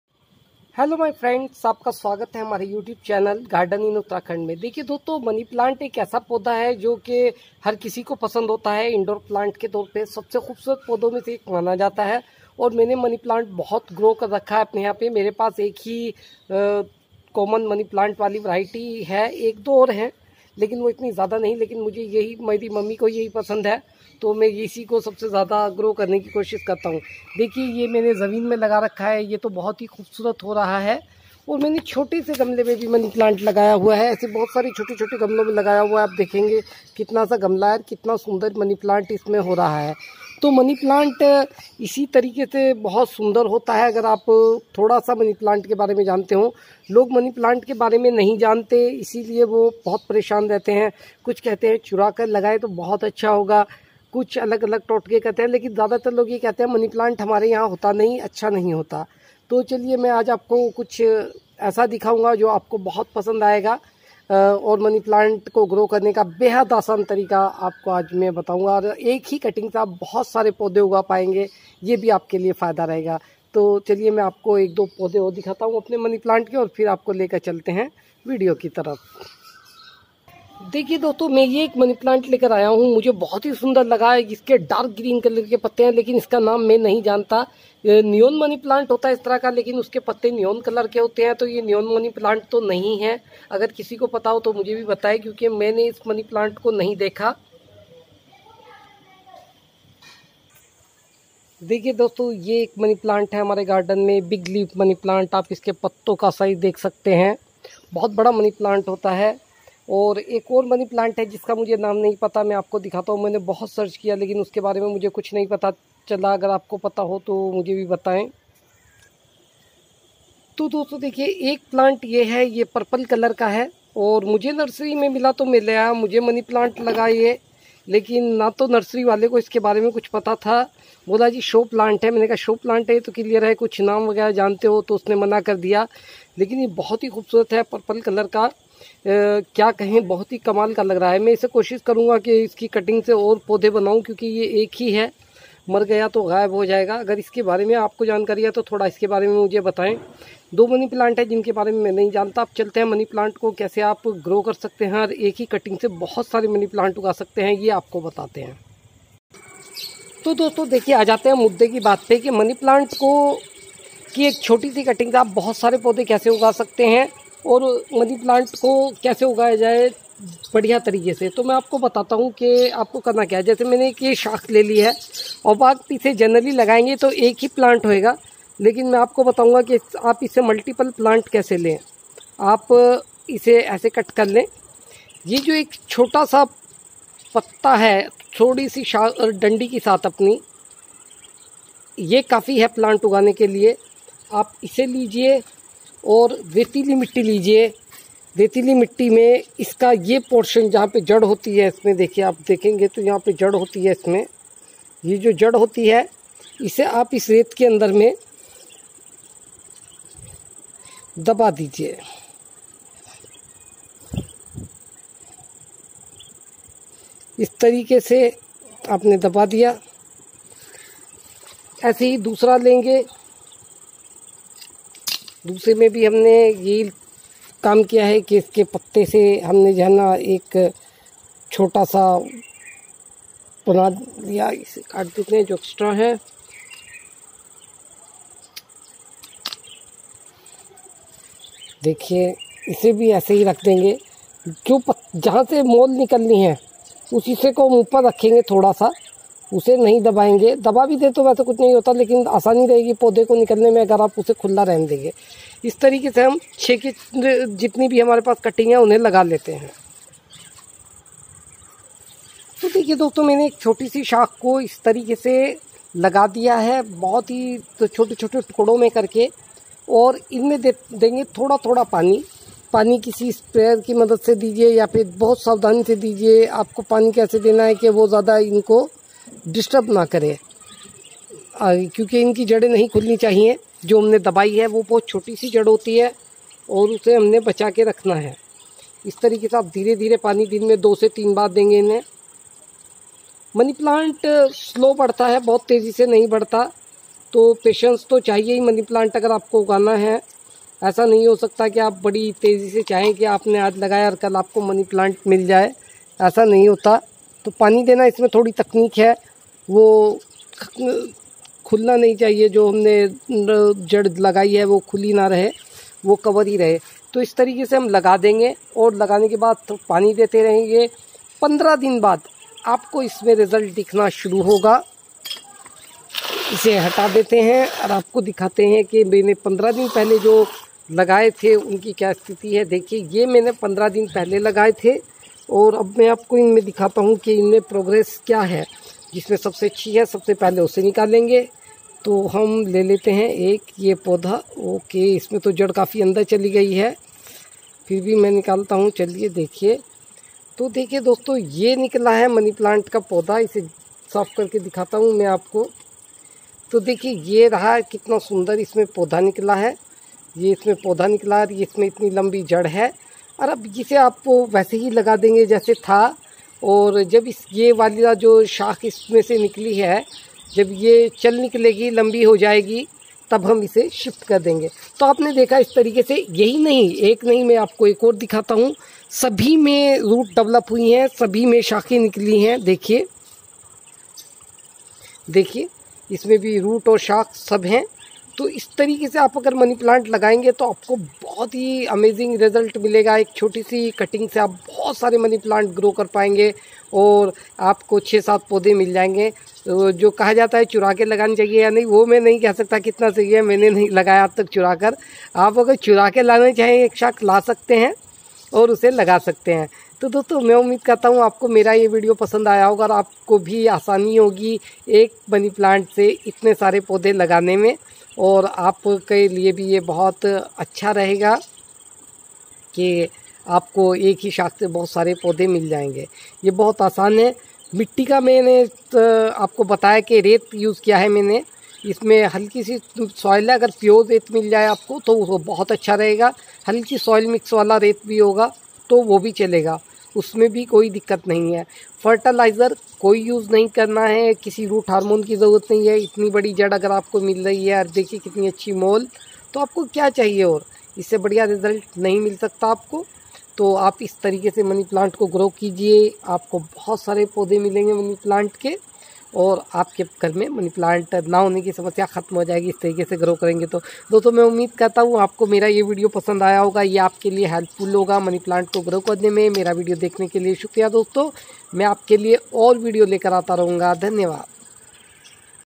money plant उगने का अनोखा तरीका | एक cutting से उगाये सेकड़ो plants.
money plant को उगने के एक ऐसे तरीके को में आज बताऊंगा जिससे आप बड़ी आसानी से मनी प्लांट के अनेक पौधे एक छोटी सी cutting से ही उगा पायेंगे |